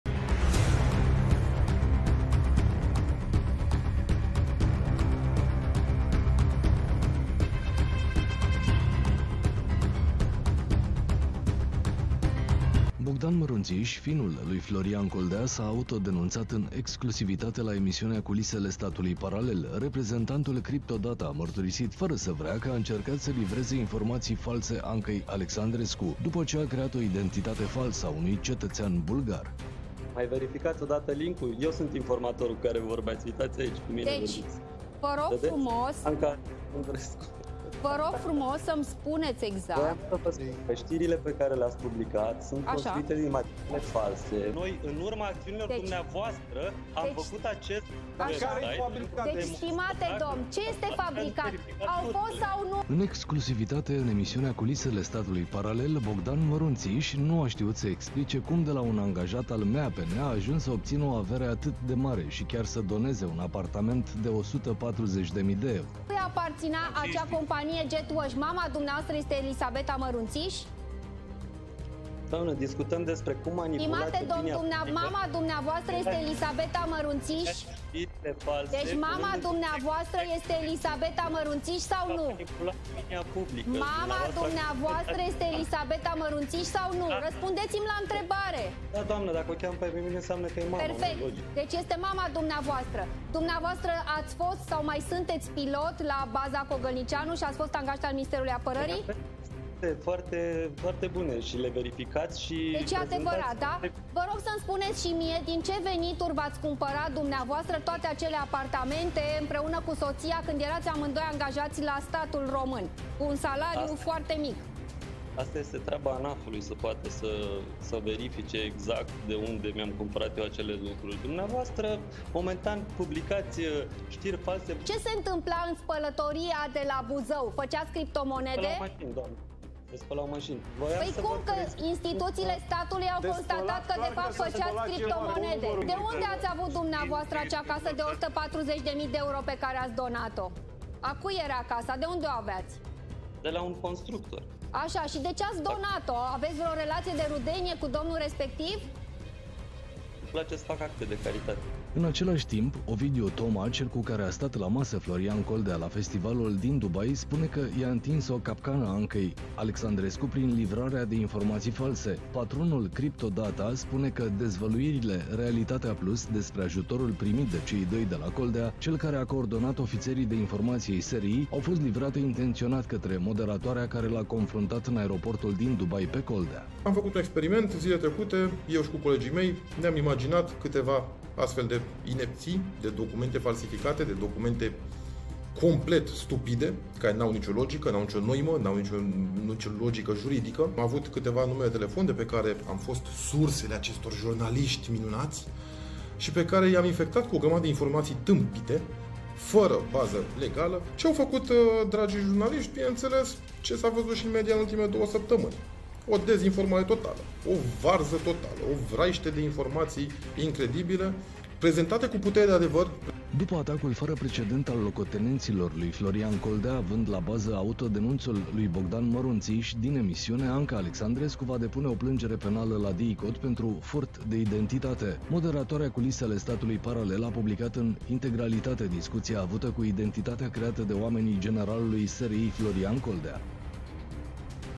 Atac fără precedent la Anca Alexandrescu! Ce pun la cale acoliții "Generalului negru"
Atac fără precedent la Anca Alexandrescu după dezvăluirile din rețeaua Coldea! Acoliții "Generalului negru" pun la cale operațiuni sub steag fals: atac la securitatea națională